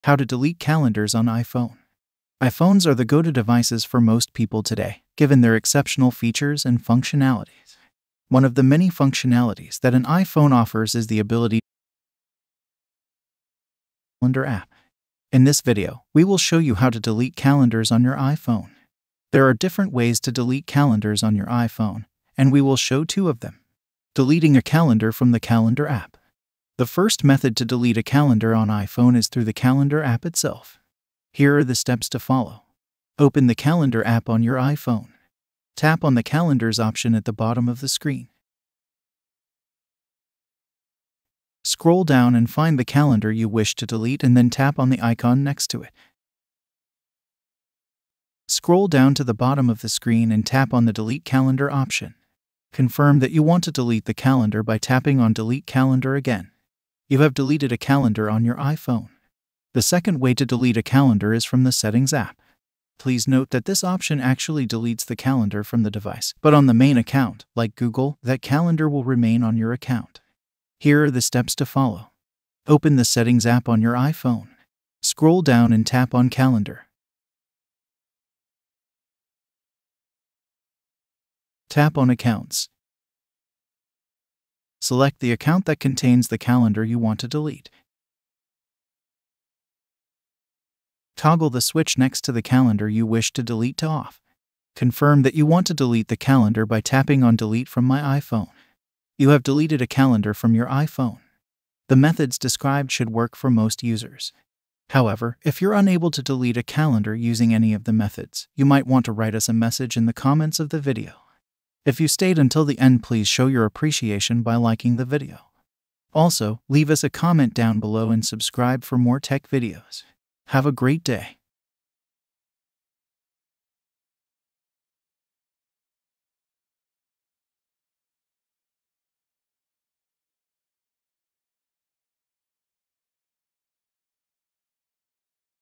How to Delete Calendars on iPhone
Is your iPhone full of appointments you don't use or don't need, making it hard to keep track of your schedule? Time to clean up! In this tutorial, we'll show you how to delete calendars from your iPhone, which will help you clean up your schedule and make it easier to follow.

Having too many schedules or ones you don't need can be confusing and make it hard to stay on top of things. With a few easy steps, you can get rid of calendars that you don't use on your iPhone. This will keep your schedule clear and easy to handle.

In this step-by-step guide, we'll show you how to delete your iPhone's plans using both built-in features and third-party apps. You'll learn how to get rid of certain calendars, stop receiving shared calendars, and keep your iPhone's plan clean and easy to use.

Learn in this tutorial How to Delete Calendars on iPhone